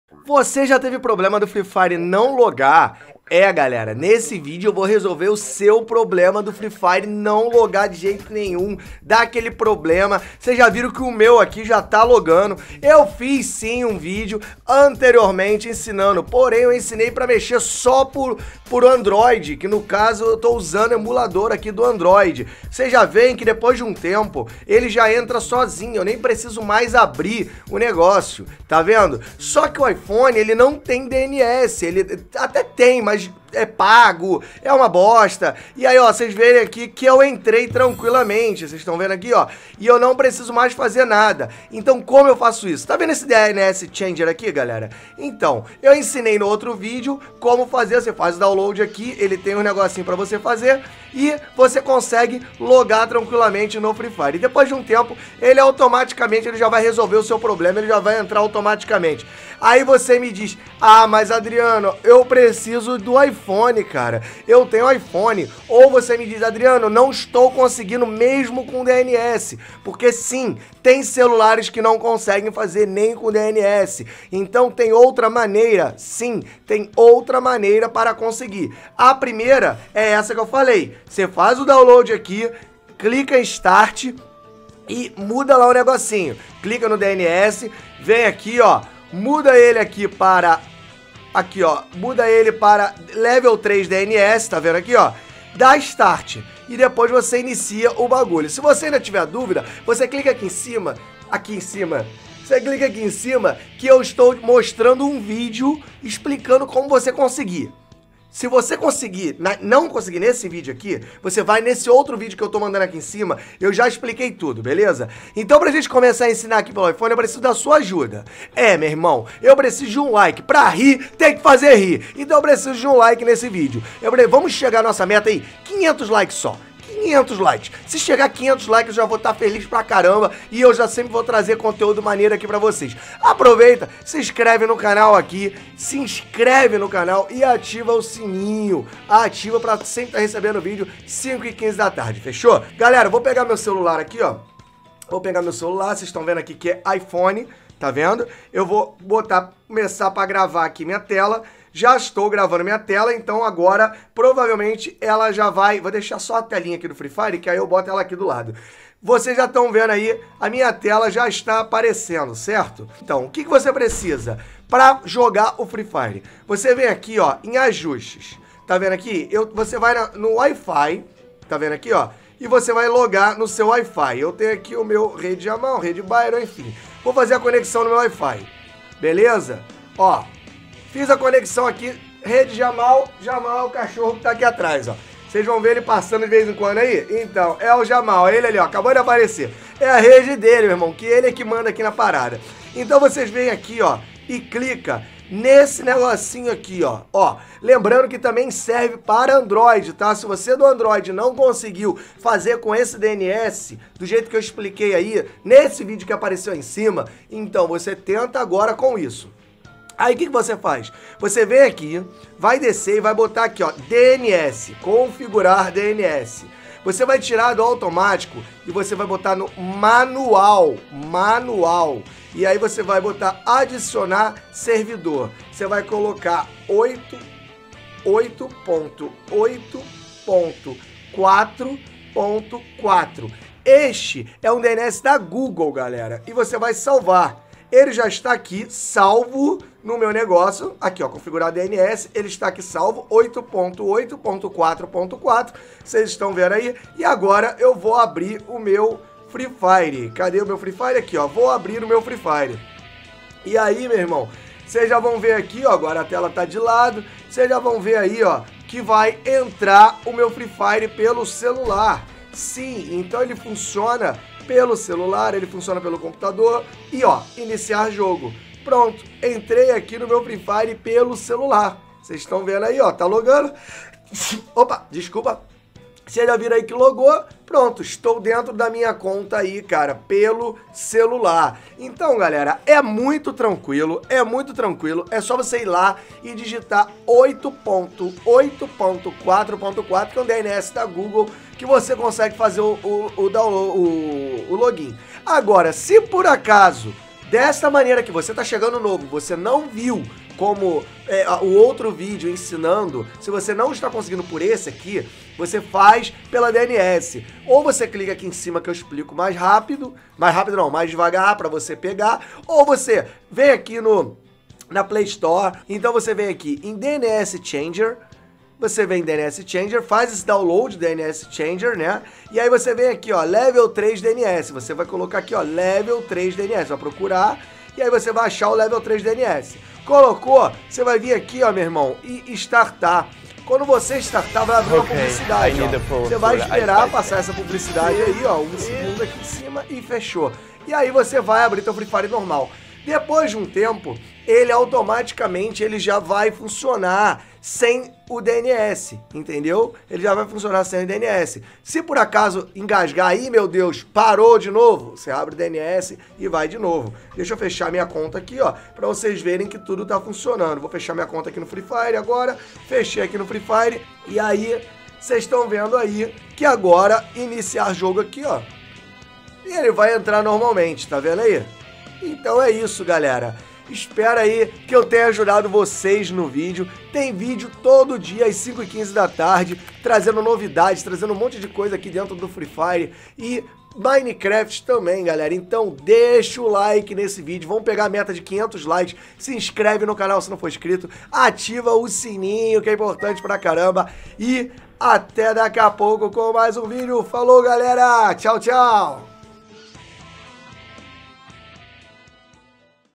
COMO RESOLVER O "ERRO DE AUTENTICAÇÃO FALHOU" DO FREE FIRE NO IPHONE E NO ANDROID!
Free Fire
Free Fire Battlegrounds

⍣ "Eu gosto do impossível porque lá a concorrência é menor." ⍣